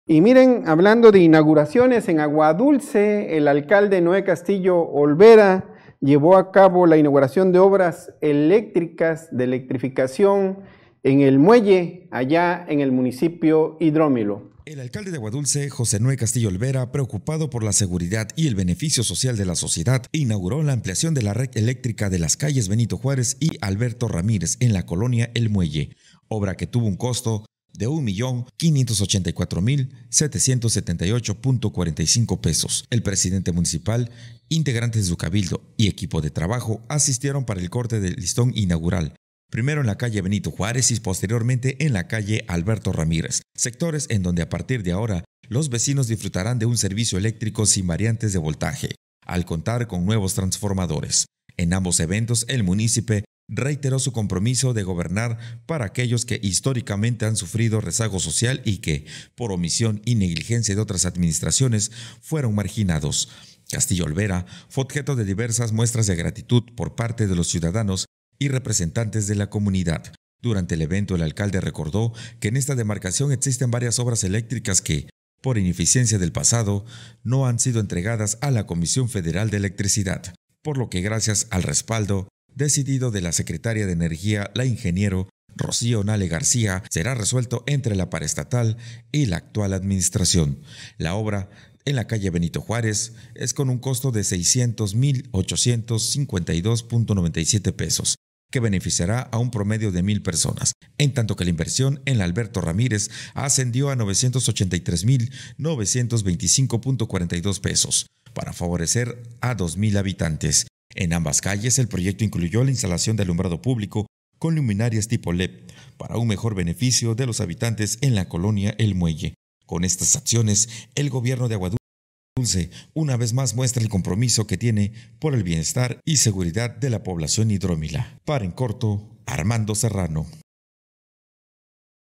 INAUGURA ALCALDE OBRAS ELÉCTRICAS EN EL MUELLE#EnCorto
El alcalde de Agua Dulce, José Noé Castillo Olvera, preocupado por la seguridad y el beneficio social de la sociedad, inauguró la ampliación de la red eléctrica de las calles Benito Juárez y Alberto Ramírez, en la colonia El Muelle, obra que tuvo un costo de un millón 584 mil 778.45 pesos.